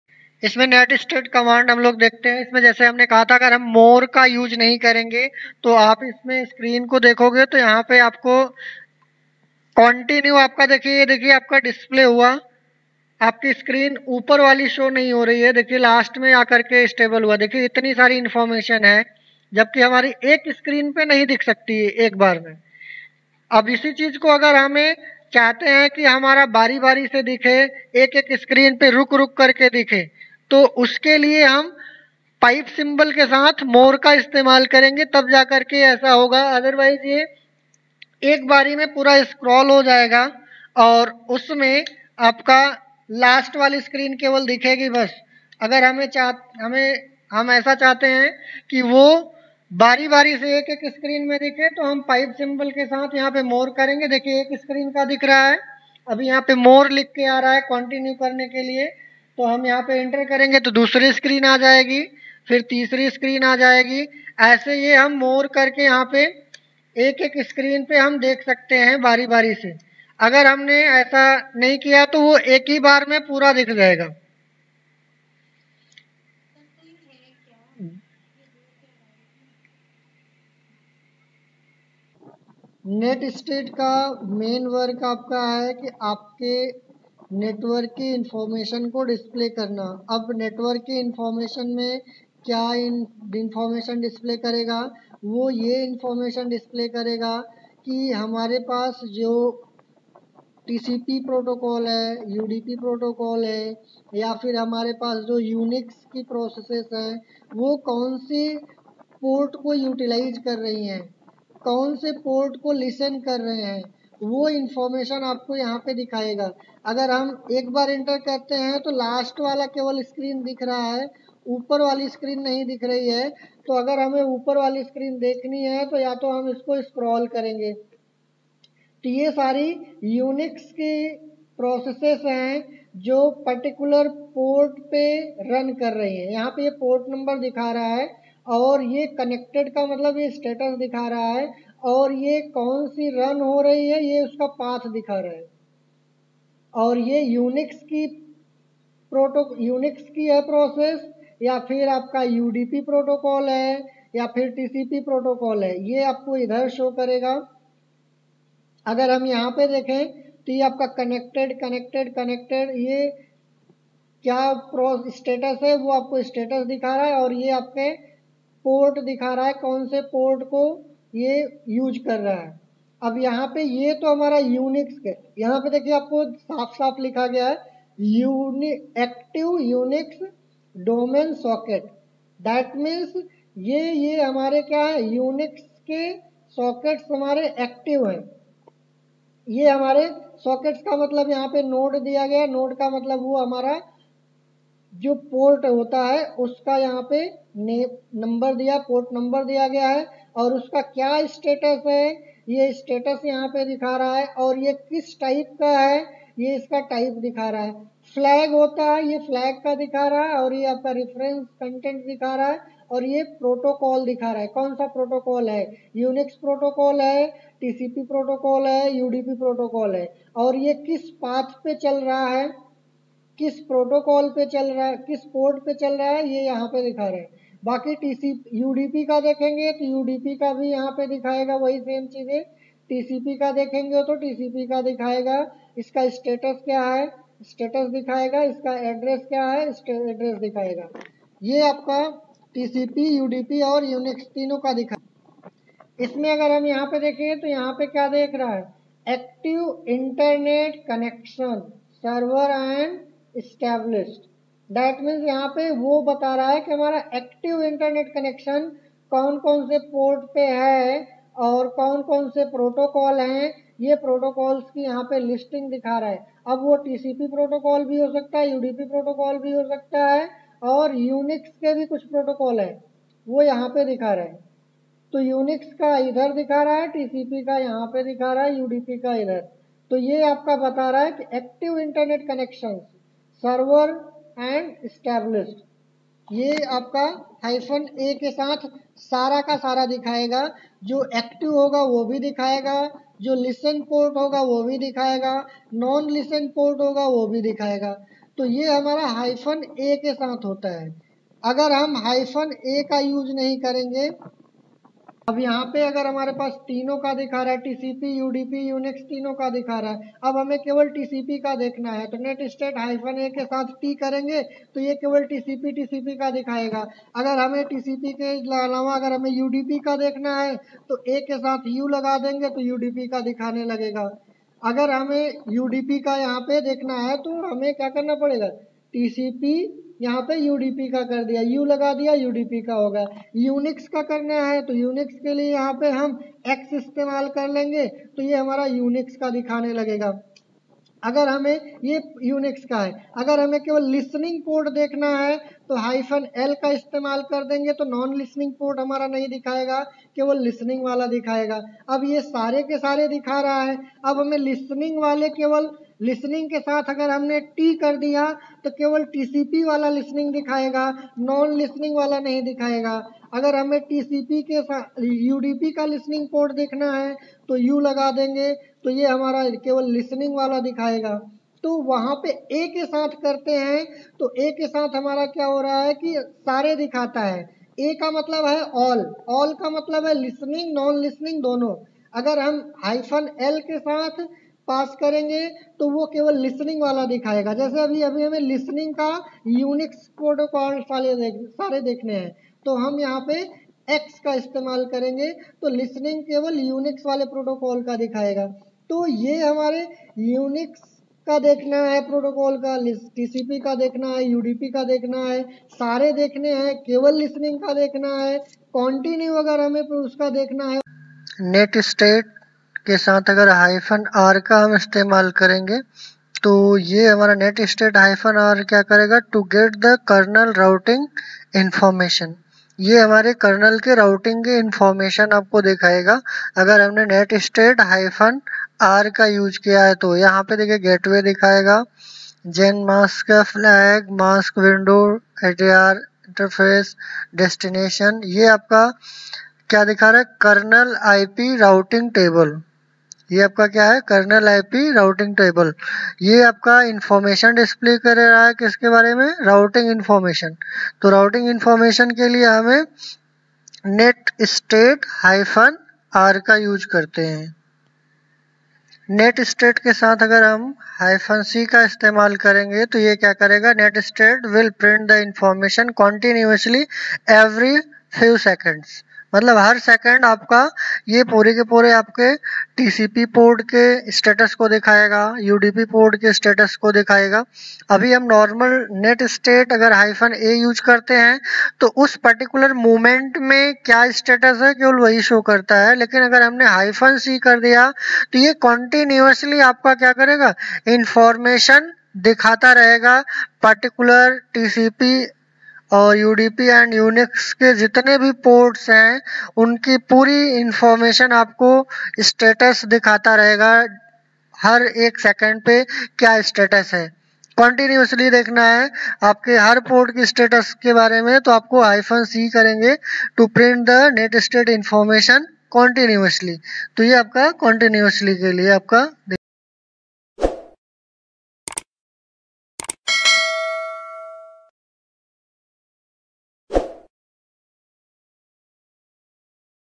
7-Network Monitoring netstat exercise | Listening and Non Listening Port status
Network Monitoring netstat exercise | Listening_port and Non Listening Port status
To get more knowledge about other professional course, Industrial training , web_designing, Web_Development, cloud, Programming, hardware_Networking and other job_oriented course with basic to advance.